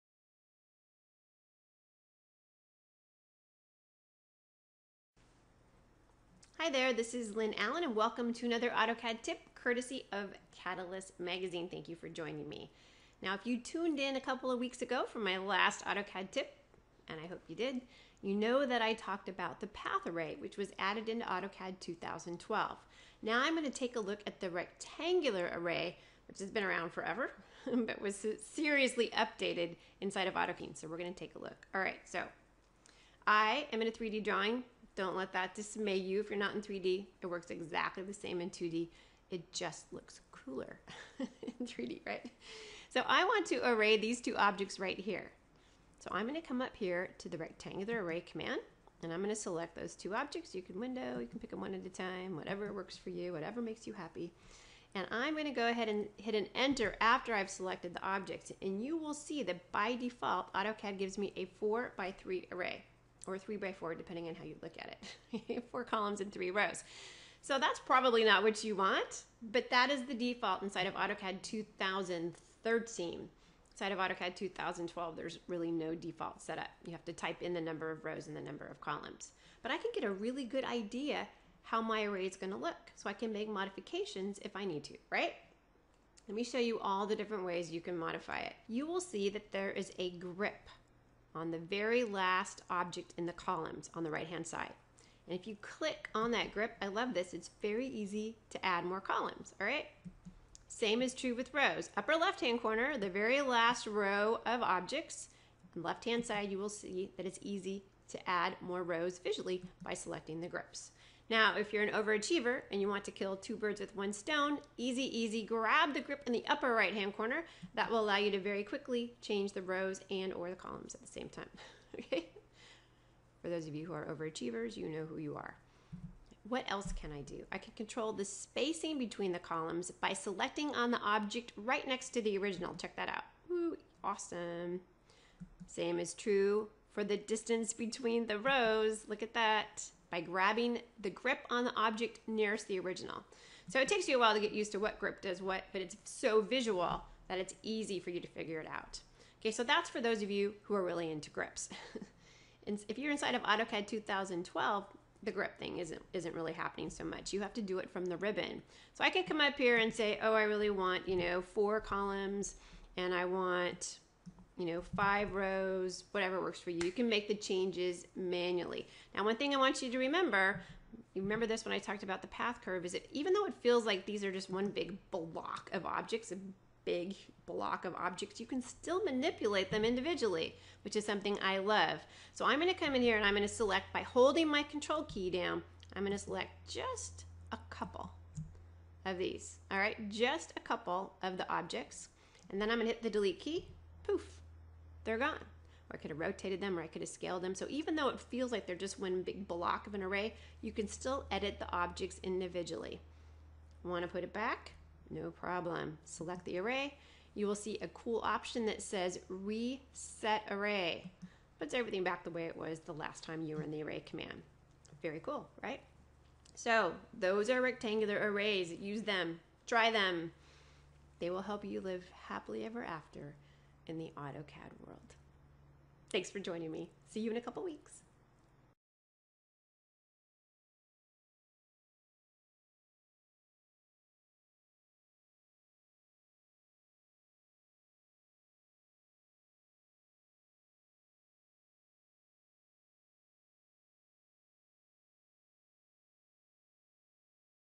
Create Rectangular AutoCAD Arrays in a Hurry (Lynn Allen/Cadalyst Magazine)
The updated array tools make it easier than ever to create speedy rectangular arrays. In this video tip from Cadalyst and Lynn Allen, you'll learn the ins and outs of creating and editing rectangular arrays for maximum productivity. (Available in standard AutoCAD since 2013.)